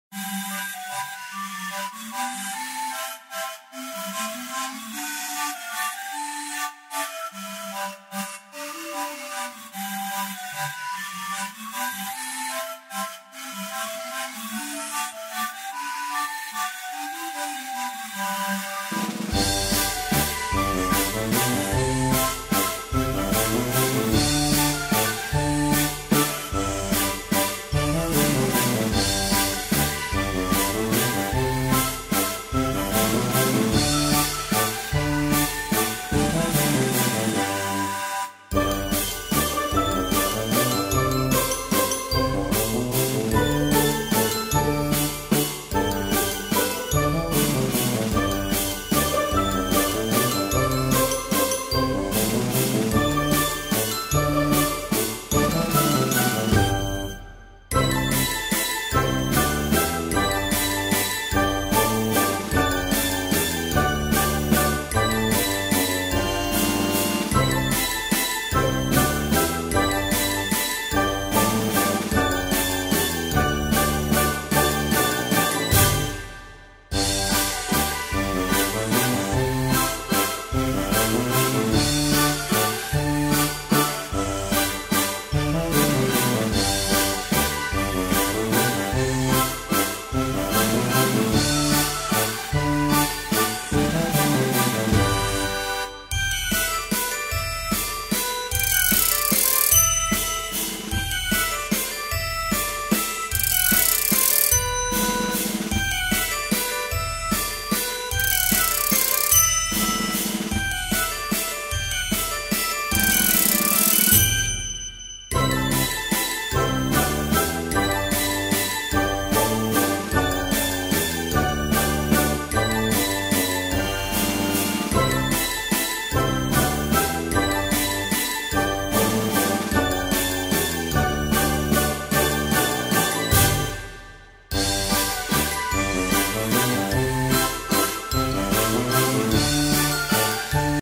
Frankfurter Pferdekarussell - Roie @ Wiesbadener Wilhelmstraßenfest - Theatrium 2013
Das "Frankfurter Pferdekarussell" von der Fa. Roie auf dem Wiesbadener Theatrium (Wilhelmstraßenfest) am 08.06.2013 (Offridevideo)

Besitzer: L. Roie, Frankfurt
Typ: Doppelstock-Bodenkarussell
Hersteller: Bertazzon (I)
Baujahr: 1989